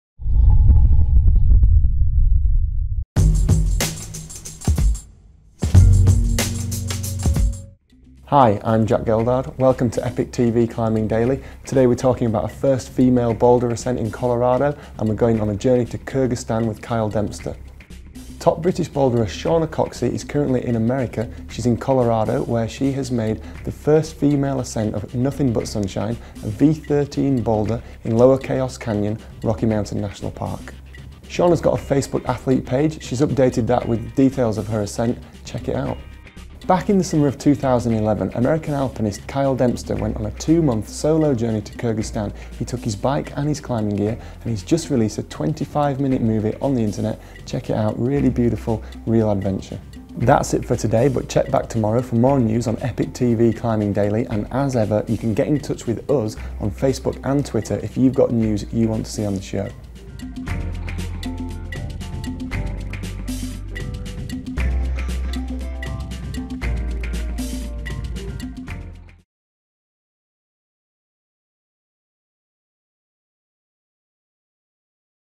A First Female Boulder Ascent in Rocky Mountains - EpicTV Climbing Daily
Today's report follows English climber Shauna Coxsey in Rocky Mountain National Park, a first female boulder ascent of a V13 boulder.

Kyle Dempster has made a movie of his 2011 2-month solo journey in Kyrgyzstan.